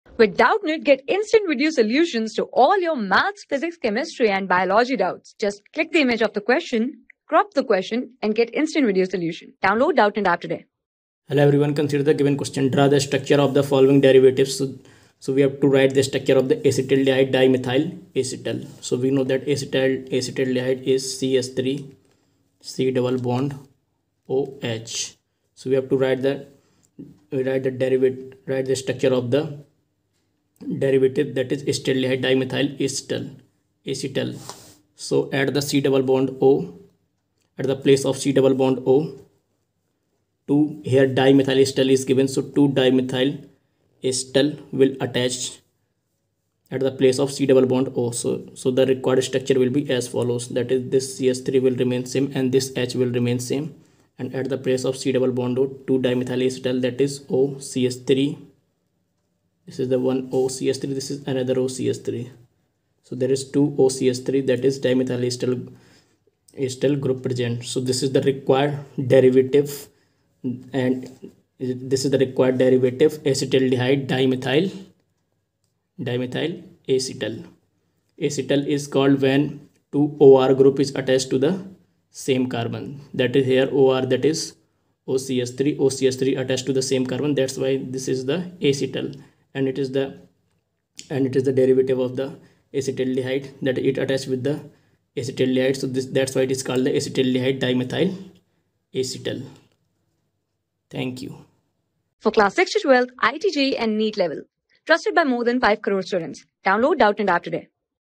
Draw the structures of the following derivatives :Acetaldehyde dimethyl acetal | 12 | ALDEHYDES...
Draw the structures of the following derivatives :Acetaldehyde dimethyl acetal
Class: 12
Subject: CHEMISTRY
Chapter: ALDEHYDES, KETONES AND CARBOXYLIC ACIDS
Board:ICSE

👉 Have Any Query? Ask Us.